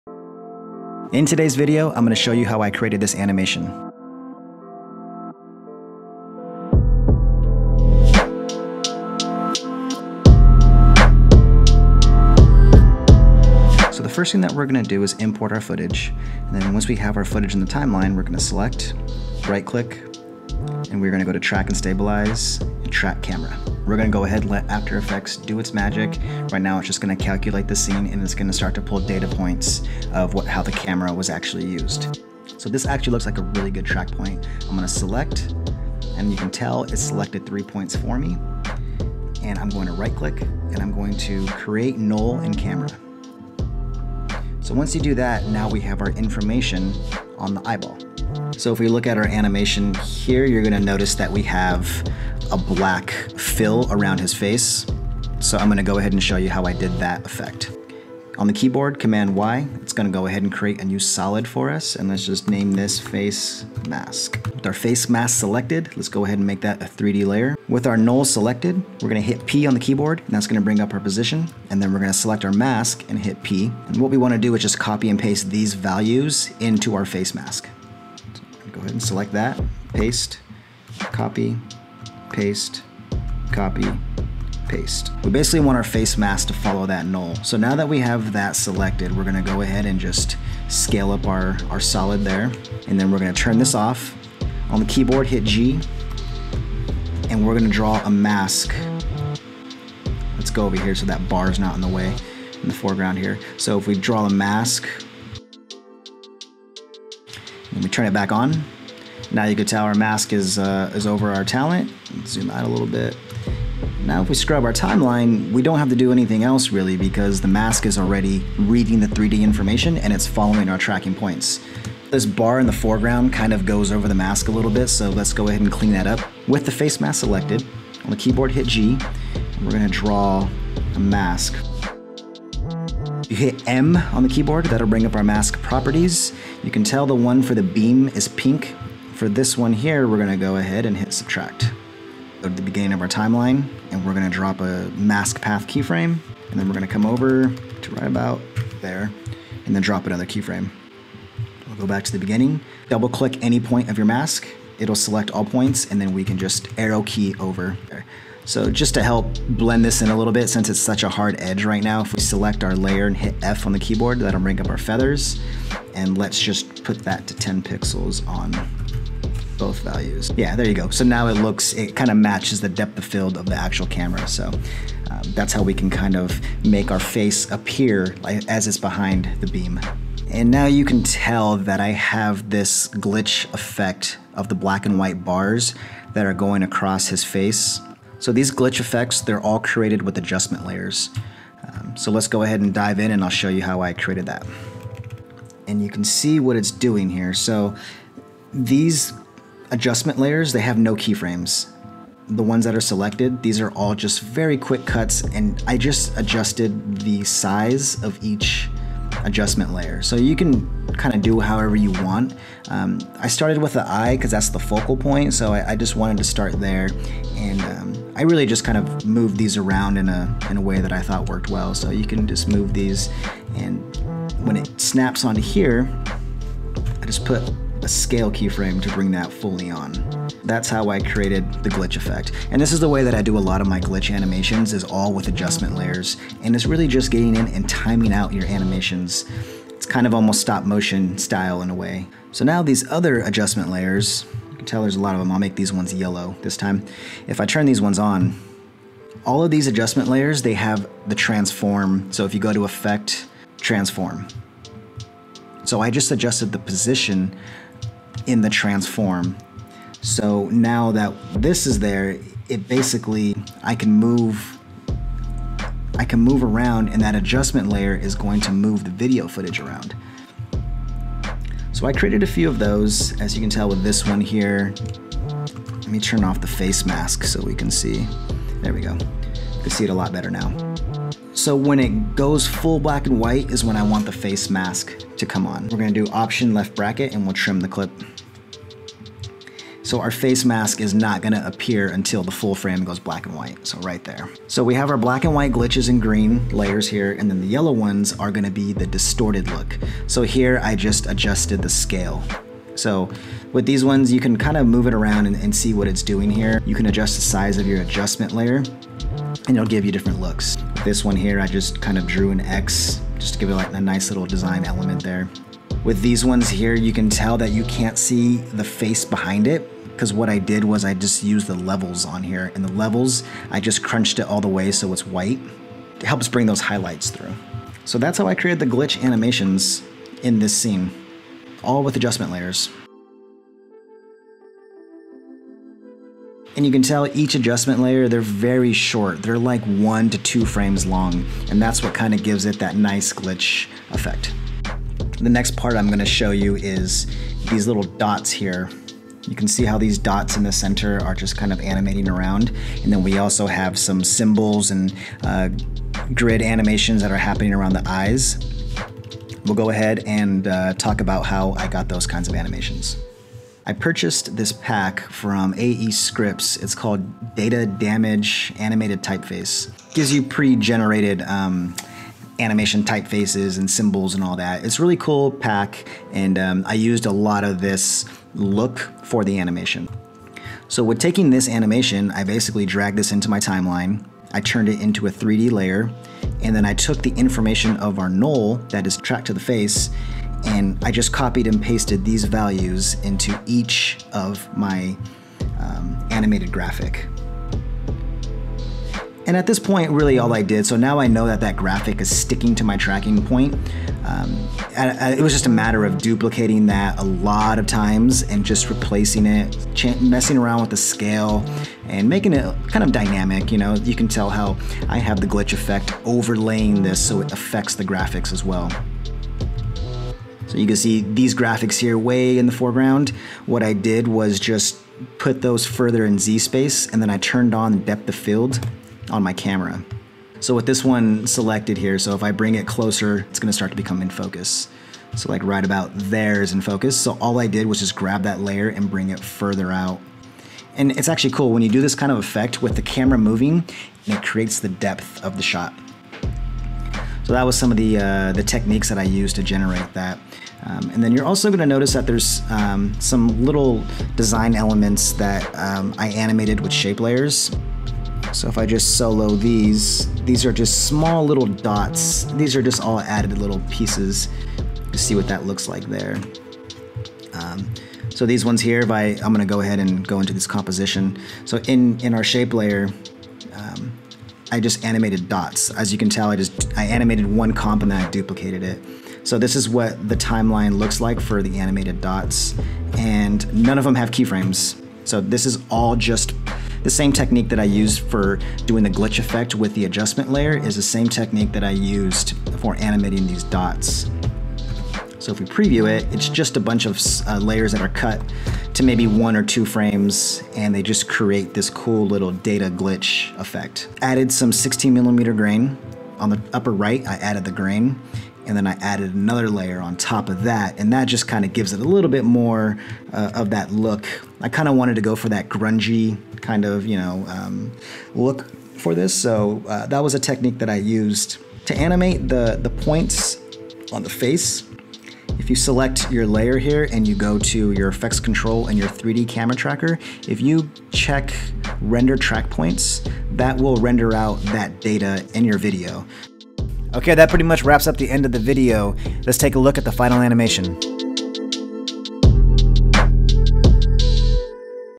How to create GLITCH ANIMATIONS with Motion Tracking in After Effects tutorial
In this After Effects tutorial I break down how I create glitch animations, we take it a step further and add some motion tracking as well. This tutorial is jam-packed with info and I hope it helps take your videos to the next level. Feel free to drop comments/questions below! Happy glitch’n

Main video camera
Mic for camera
Main lens I use
Mavic Pro
iPhone Gimbal
Tiny tri pod
Normal Tripod
Monitor for Camera
Main Camera Bag
Photography Camera
Favorite prime lens